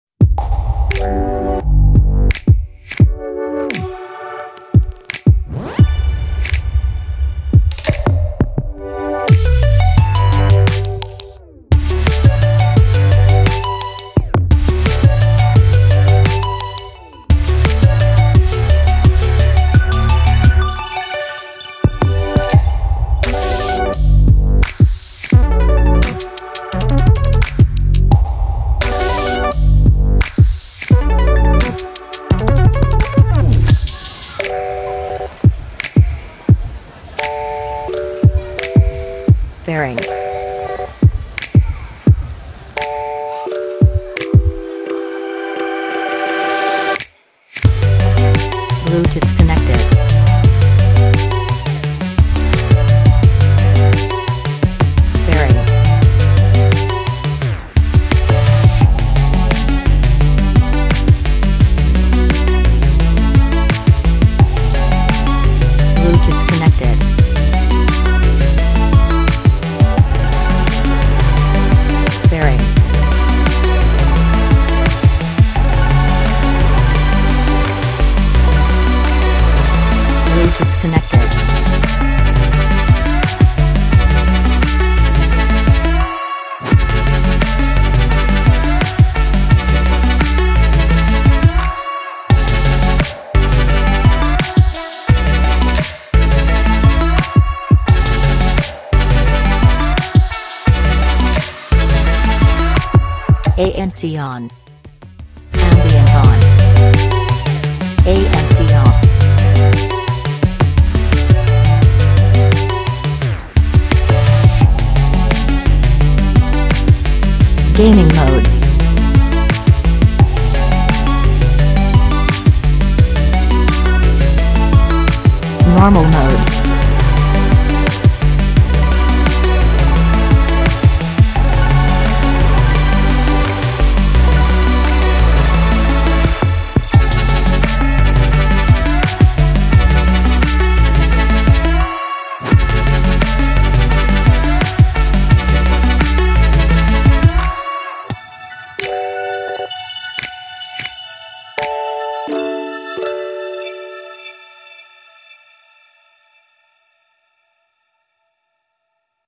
Razer Opus X | Instructional Video
Whether you’re jamming to your favorite tunes, watching a movie, or gaming, find out how you can optimize your audio experience with the Razer Opus X through this instructional video.